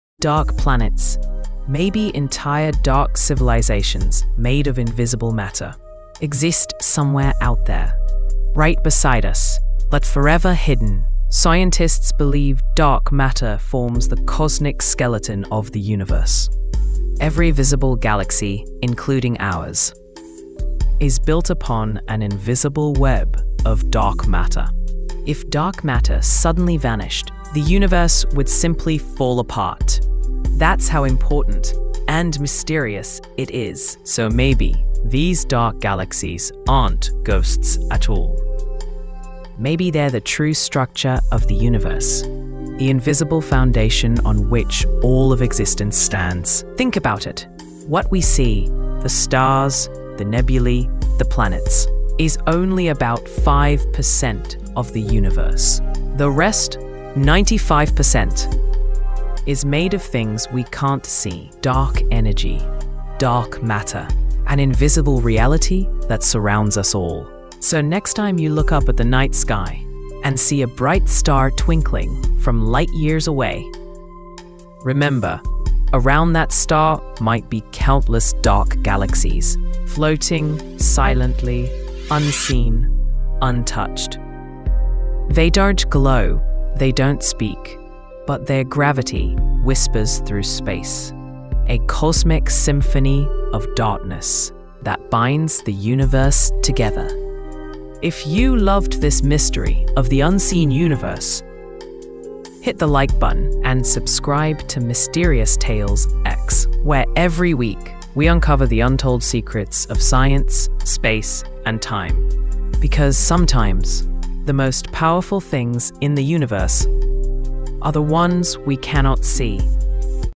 🌌 “Dark Matter Galaxy – Where Even Light Cannot Reach!” 🌌
In the endless darkness of space, there exists a mysterious galaxy —
a place where not a single star shines,
where even light itself cannot reach. 🌑

Scientists call it a “Dark Matter Galaxy” —
a region made almost entirely of something invisible…
something that neither emits nor reflects light.

Is this galaxy built purely from dark matter?
Could there be an unknown universe hidden within it?
Or are these just cosmic shadows trying to tell us something?

Watch this video to uncover the truth about
the galaxies of darkness,
where even light disappears. ✨